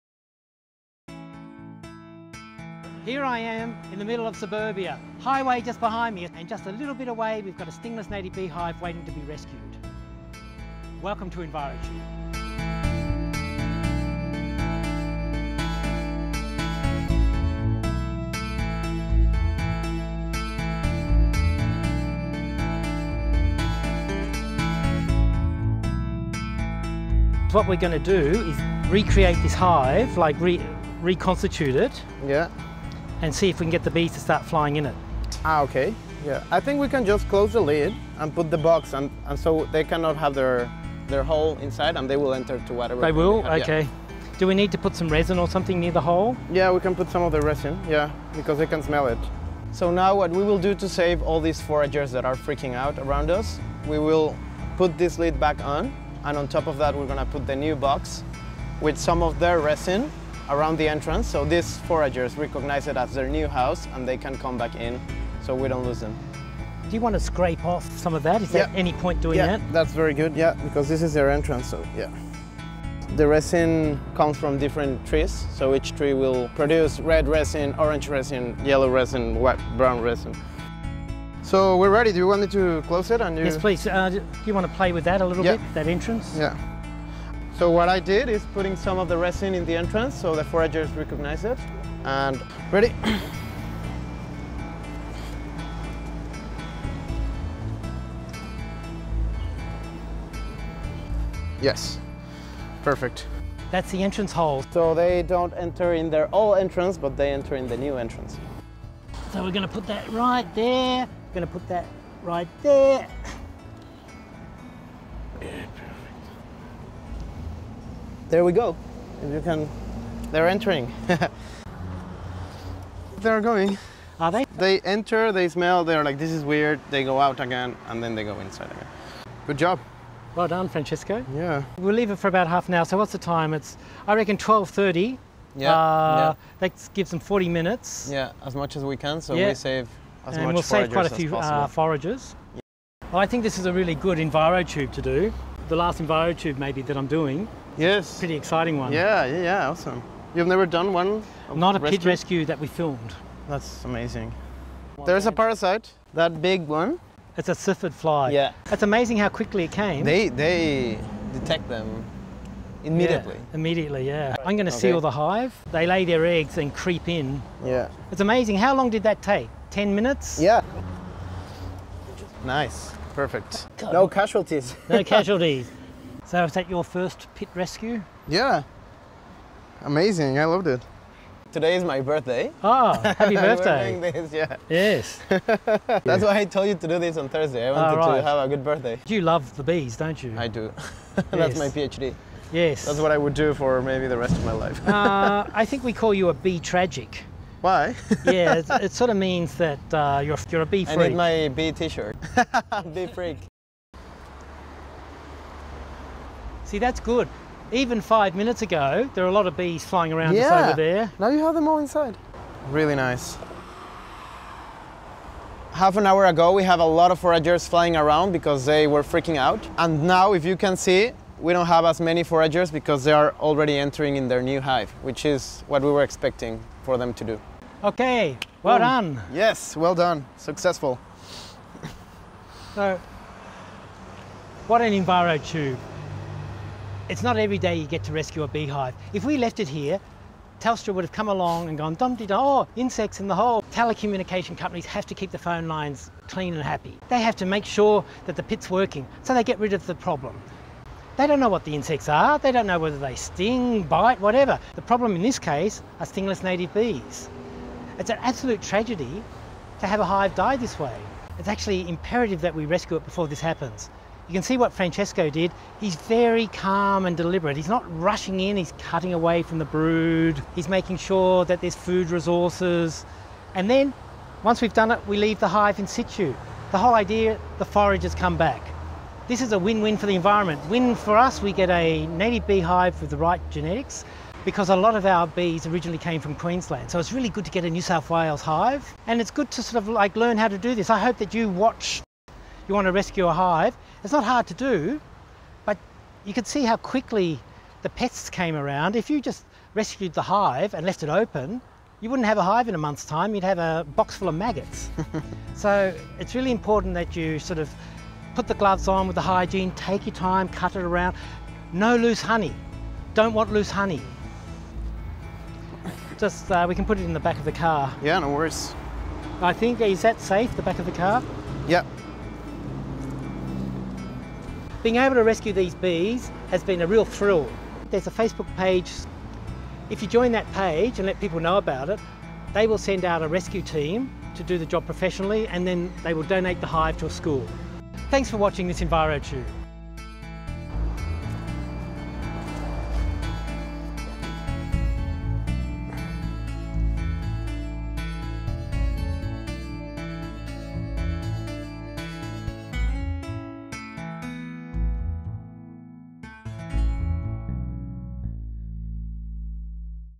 Bees Underground? In telecommuniction Pit - Envirotube
Bee Rescue Ep 3 - Envirotube presenter Peter Clarke teams up with PhD candidate, Francisco Garcia Bulle Bueno, from Mexico. Together they demonstrate how to rescue a native bee hive from a communications pit. In this 3rd episode they successful lure the native stingless bees into the box but immediately a syrphid fly turns up and want to upset their hard work? Director, Howard Jackson, Wildhoop Productions